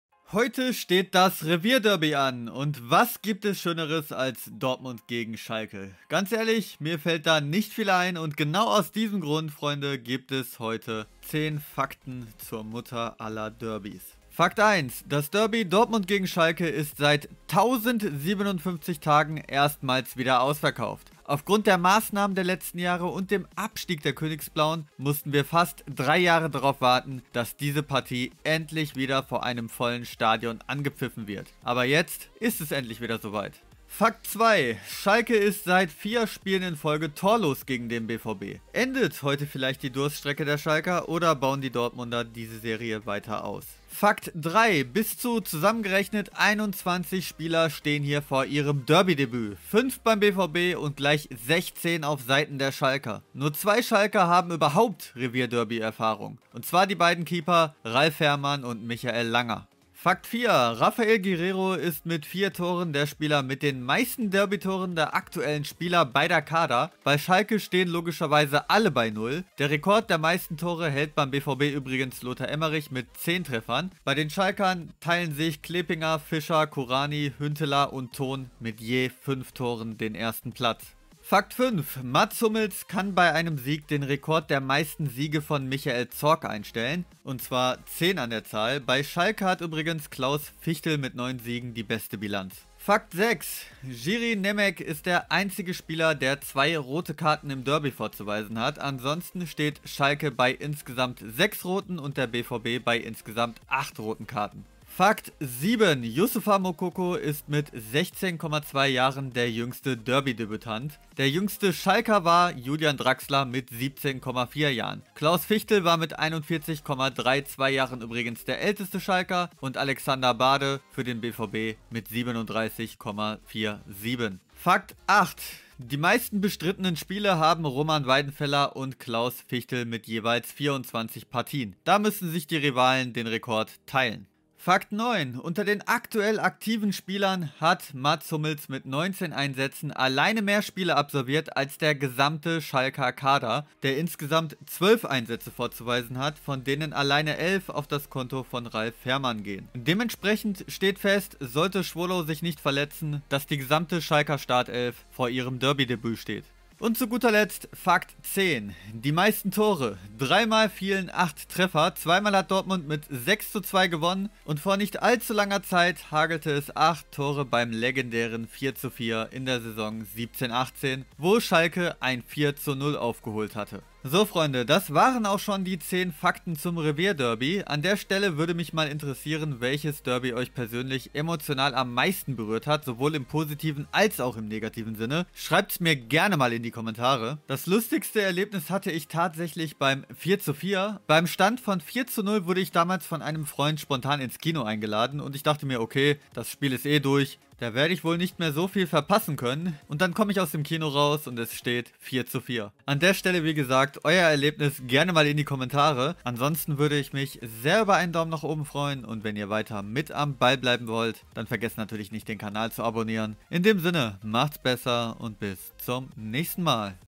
Dortmund gegen Schalke: 10 Fakten zum Revierderby!
Das Derby Dortmund gegen Schalke am 7. Spieltag 2022 steht vor der Tür und zur feier des Tages werfen wir im heutigen Video einen Blick auf 10 Fakten über das Ruhrpott Derby Borussia Dortmund - FC Schalke 04